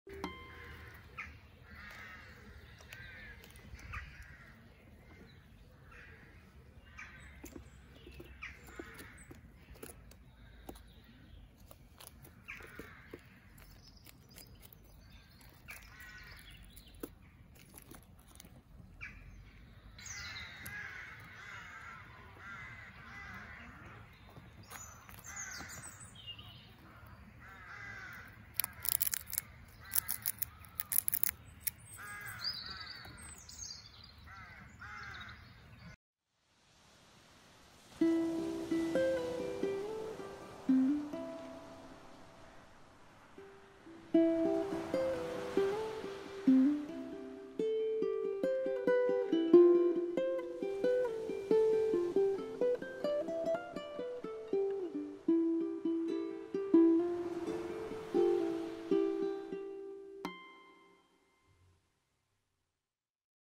The Sensational Squirrels of Tehidy Woods (ideal for cats & dogs!)
Watch as the lively squirrels of Tehidy Woods dash around, jump between branches, and explore their forest home. Tehidy Woods, nestled in Cornwall, is home to these playful creatures, offering a unique glimpse into their active lives as they forage, play, and interact in this beautiful woodland setting.

Did you know that squirrels are natural acrobats? Their bushy tails help them balance as they leap from branch to branch, and they can even rotate their ankles 180 degrees to climb down trees headfirst! These clever animals can remember the locations of hundreds of buried acorns to last through winter, showing their amazing memory and planning skills. Squirrels are also quick learners, often using teamwork and adapting their habits to stay safe and find food.

Join us for a relaxing moment with these captivating animals as they bring Tehidy Woods to life. Perfect for unwinding, connecting with nature, or enjoying a bit of woodland wonder, this video invites you to witness the charm and agility of Tehidy's delightful squirrel residents